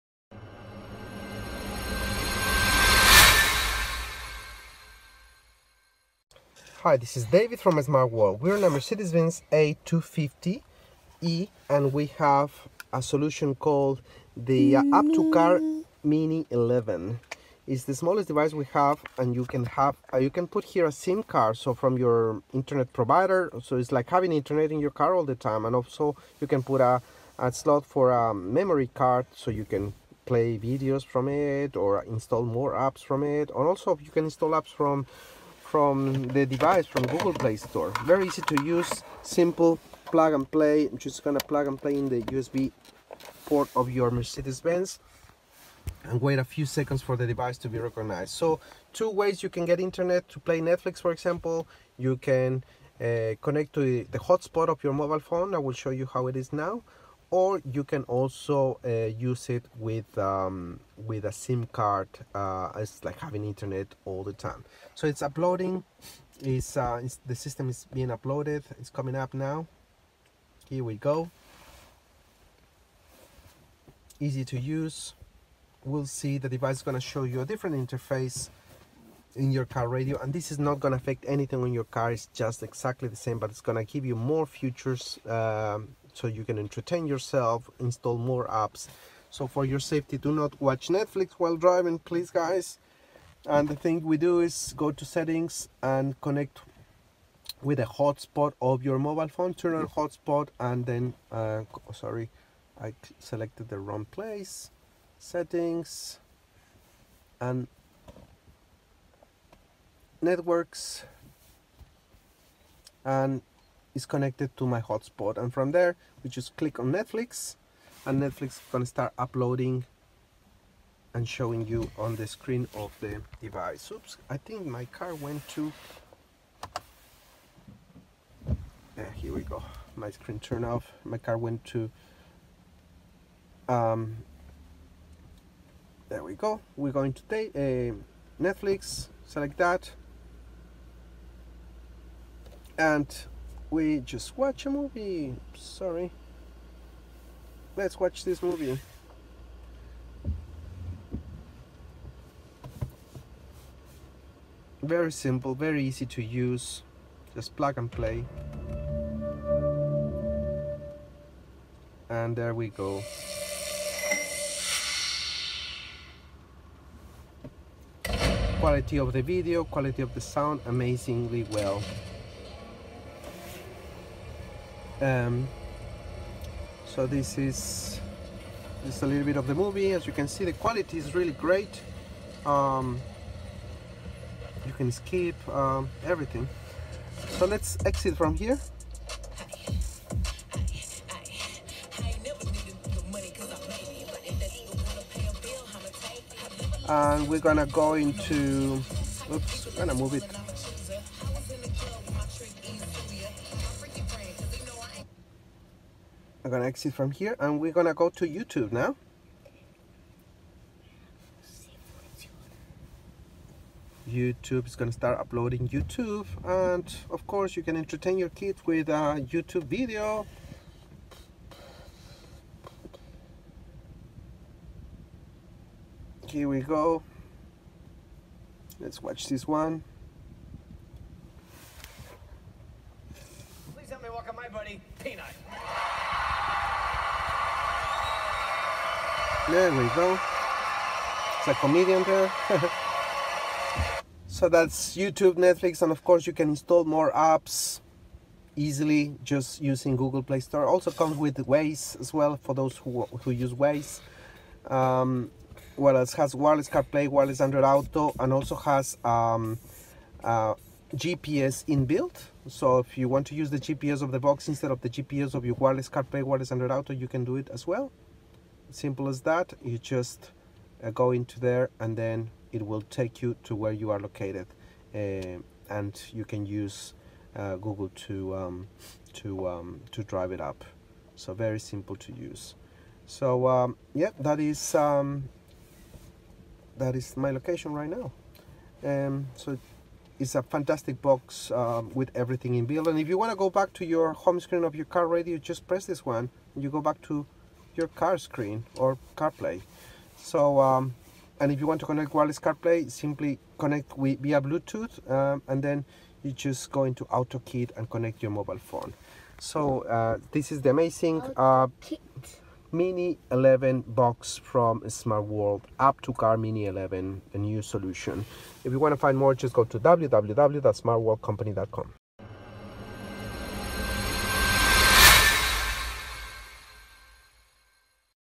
How to install Netflix and Youtube apps on Mercedes Benz A250 using App2car Mini 11 media box
App2car mini 11 the most economic media box that expands functionality of your car media head unit by installing an Android 11 OS you can download any app an your car radio watch videos, check social media and so on. For more info please visit: